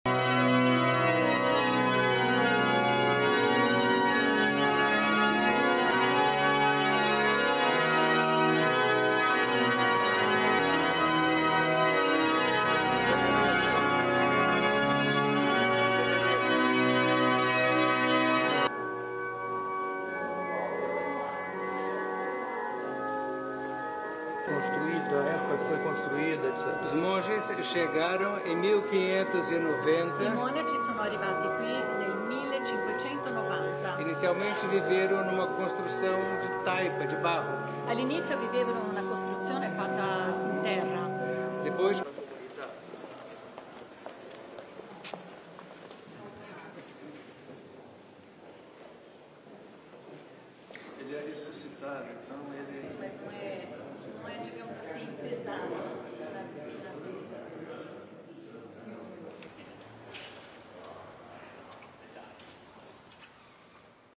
Il Presidente Mattarella visita il Monastero di San Benedetto a Rio de Janeiro
(Agenzia Vista) Il Presidente Mattarella, nel corso della sua visita alla città di Rio de Janeiro, ha visitato il Monastero di San Benedetto.